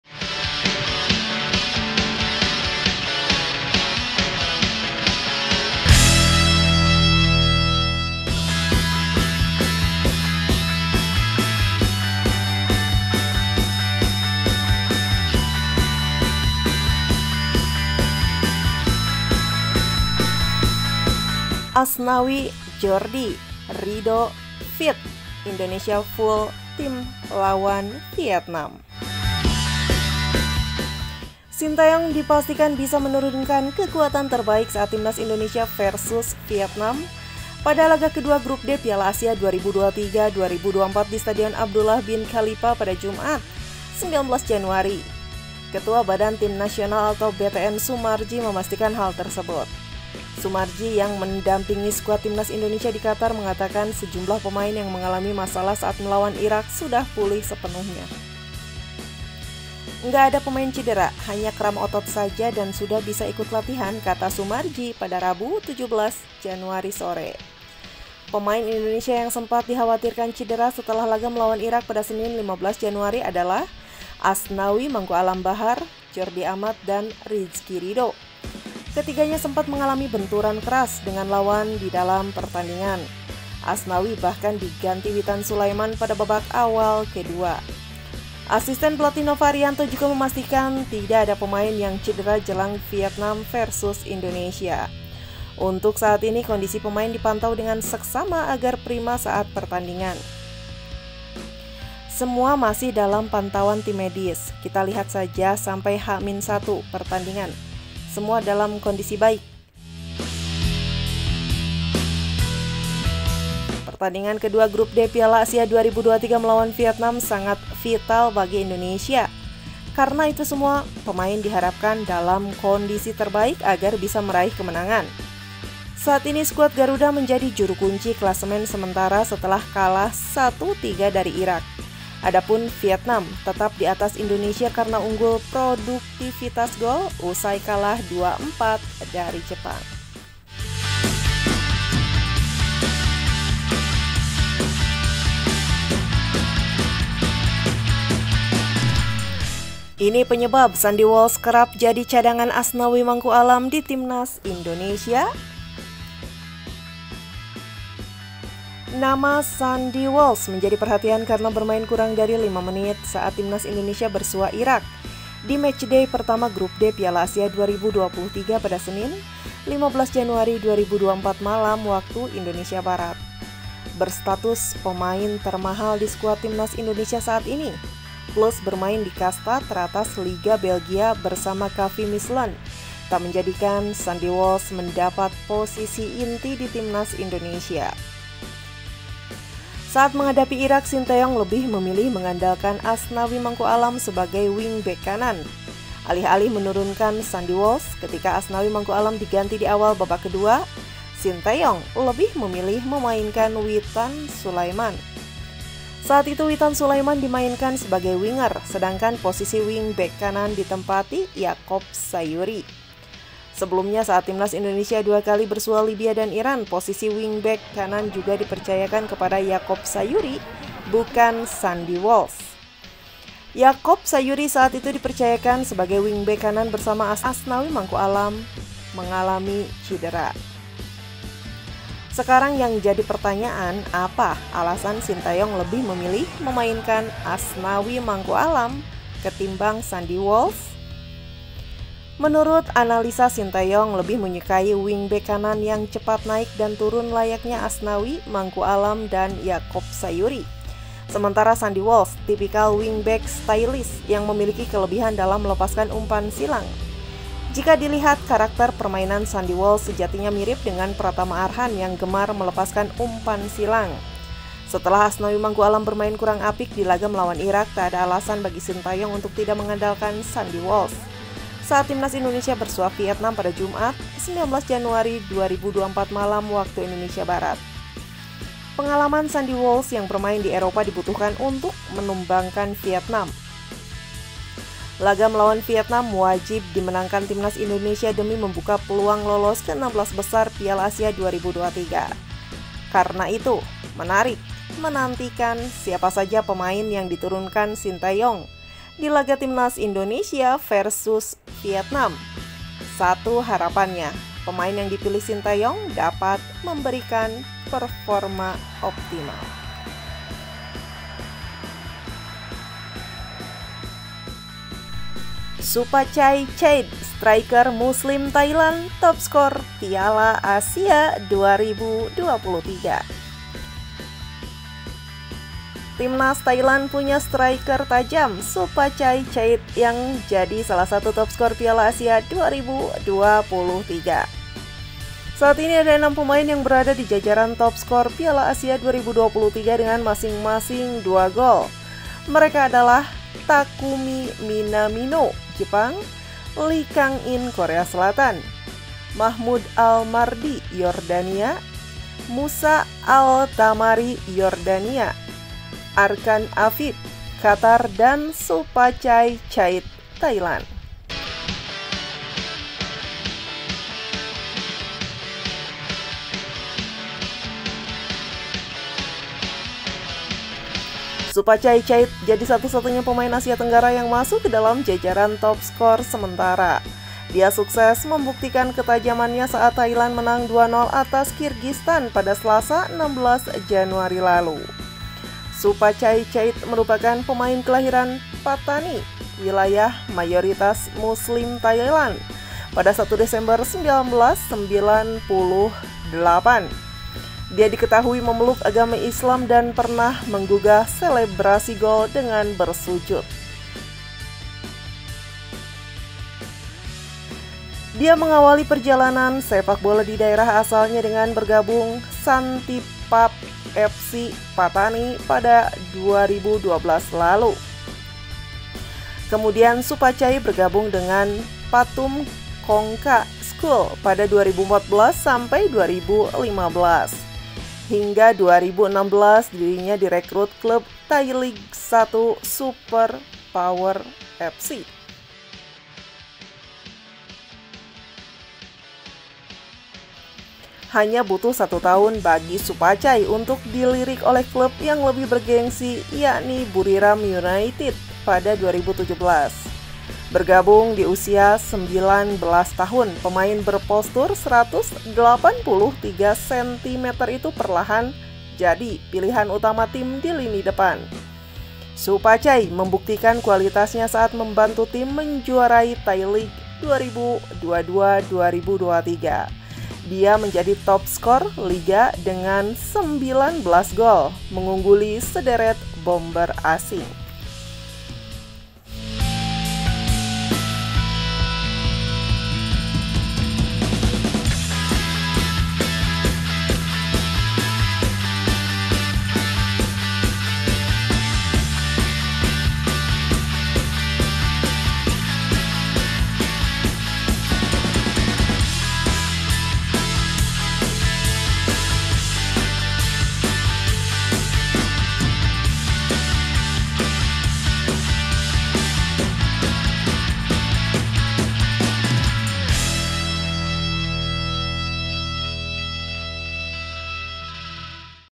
Penyebab Sandy Walsh Kerap Jadi Cadangan | Timnas Indonesia Full Team vs Vietnam | Supachai Chaided
Timnas Thailand punya striker tajam, Supachai Chaided yang jadi salah satu top skor Piala Asia 2023. Berikut profil Supachai Chaided.
Saat ini ada enam pemain yang berada di jajaran top skor Piala Asia 2023 dengan masing-masing dua gol. Mereka adalah Takumi Minamino (Jepang), Lee Kang In (Korea Selatan), Mahmoud Al Mardi (Yordania), Musa Al Taamari (Yordania), Akram Afif (Qatar), dan Supachai Chaided (Thailand).